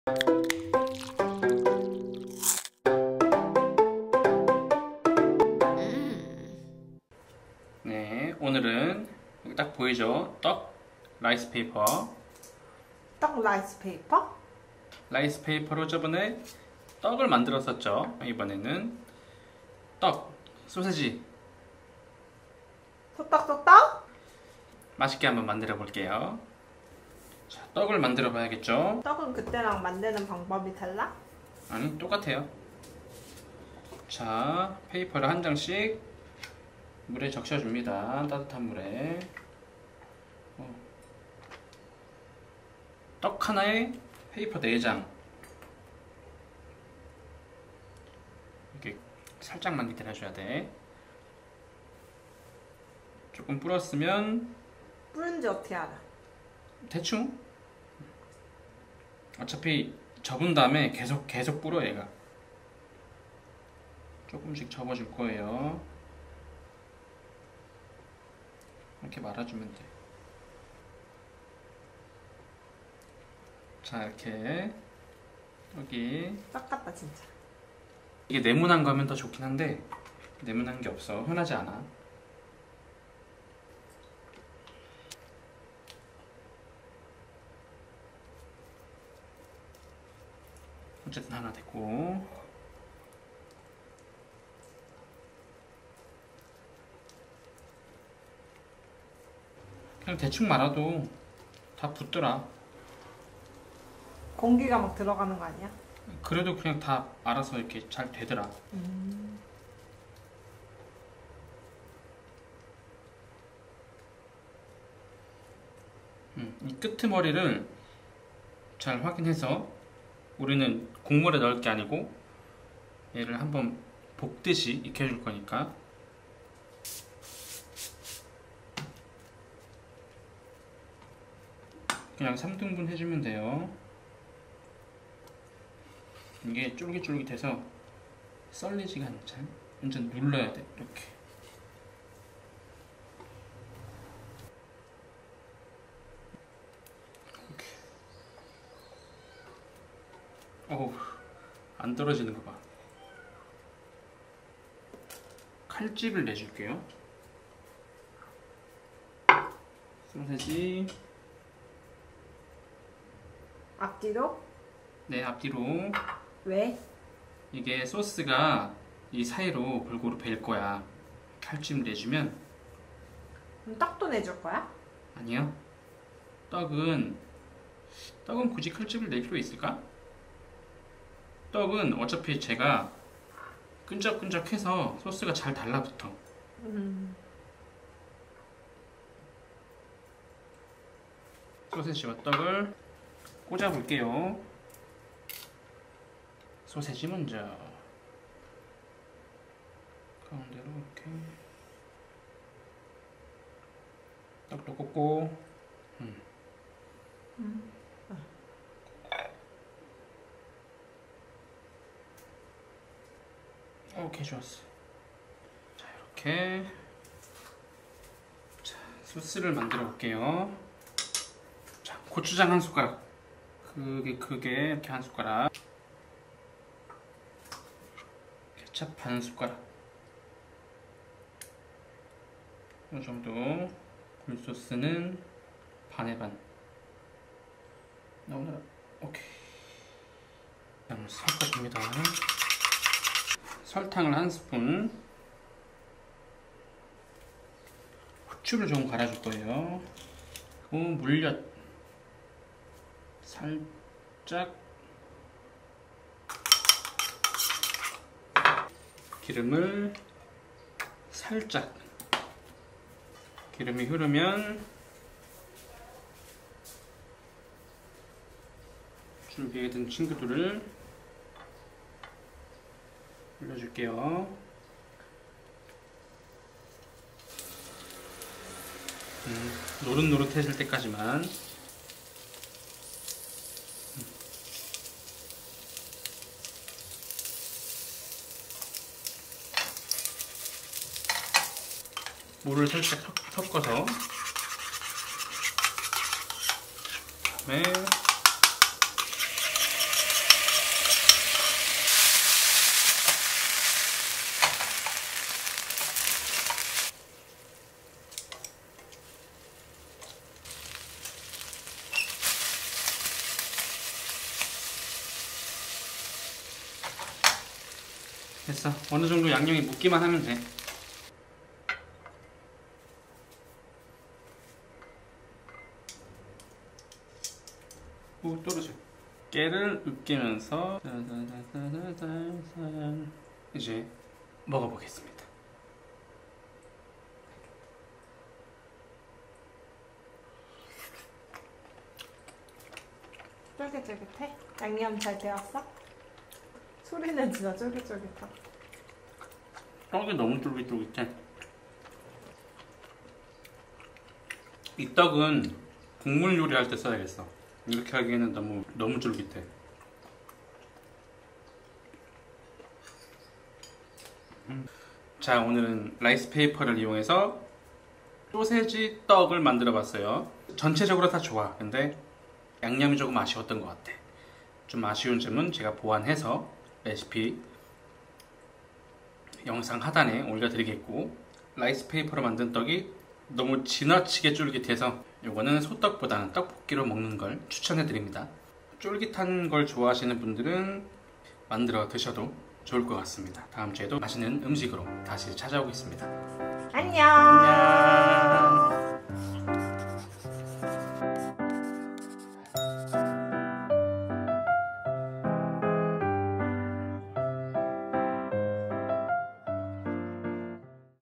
[Taechelin Guide] How To Make "Sausage Rice Cake Skewer" l Sotteok Sotteok : 소떡소떡 l Korean Food
How To Make "Sausage Rice Cake Skewer" (Sotteok Sotteok)

▶︎ Ingredients of today’s dish

Sausage (300g)
Rice platers (16)
Red pepper paste (1 tbsp)
Ketchup (30g)
Oyster sauce (15g)
Sugar and pepper (15g)

▶︎ Cooking Order

01. Soak rice paper in water and roll it to make rice cake.
02. Make a cut on the sausage.
03. Put rice cake and sausage alternately on the wooden skewers.
04. Grill rice cake and sausage moderately.
05. Apply the seasoning and bake it lightly.
06. Sprinkle sesame seeds to decorate.

How to make seasoning

Comment if you have any inquires.

▶︎ Credit
- Intro: Yoopacaca, Reviere
- LUT: Dolso cinematic LUTs